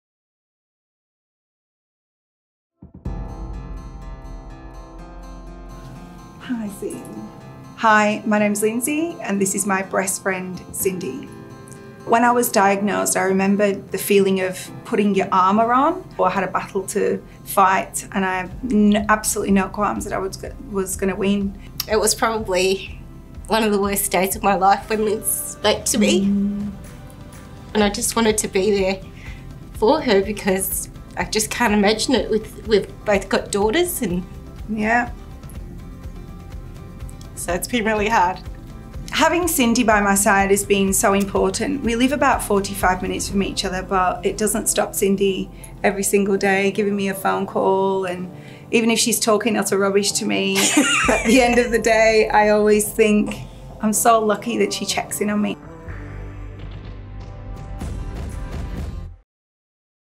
Breast Friends | Lindsey & Cindy
Meet Lindsay & Cindy 👭 These breasties have been there for each other through thick and thin. Show your support by visiting your local Bakers Delight between May 2-29.